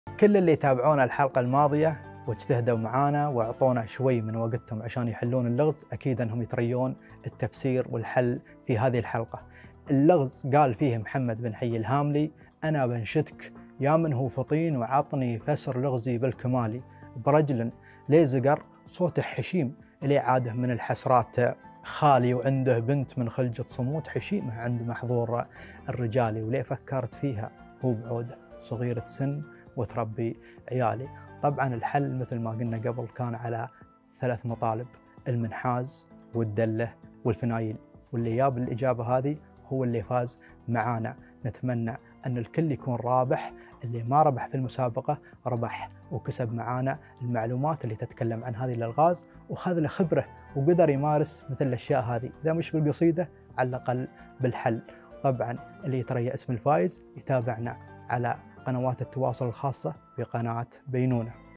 الإعلامي والشاعر عبيد بن قذلان المزروعي يكشف عن حل اللغز الشعري الخاص بالشاعر محمد بن حيي الهاملي
"أنا بنشدك يا من هو فطين
 وعطني فسر لغزي بالكمالي"